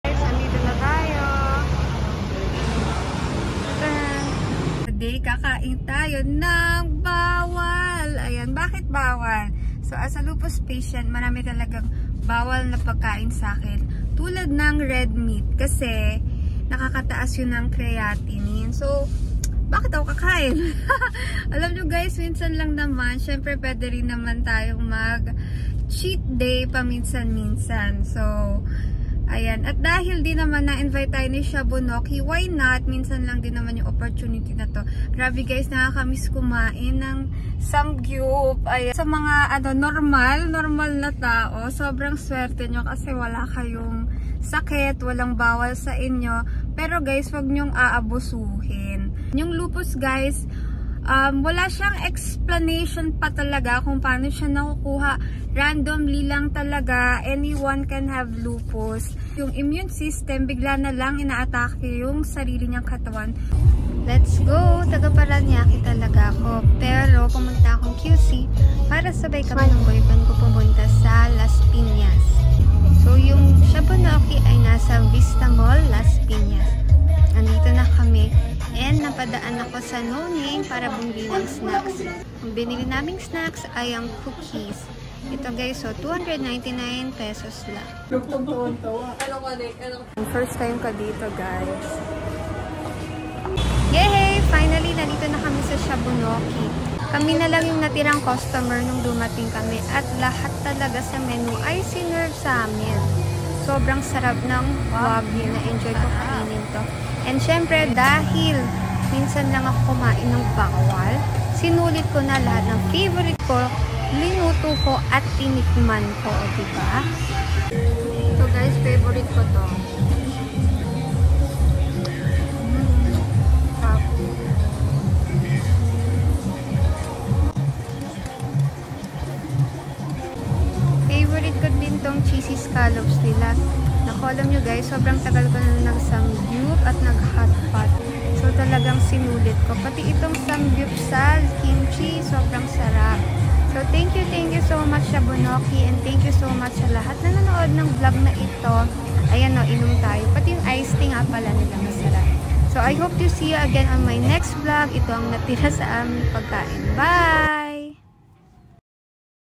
Lupus Daily: Cheat Day dahil Kakain ng Bawal + Shabunoki Mukhbanh
Hey guys thank you for tuning in to my vlog! I would like to share my struggles as a Lupus Warrior. Thank you ❤️🥹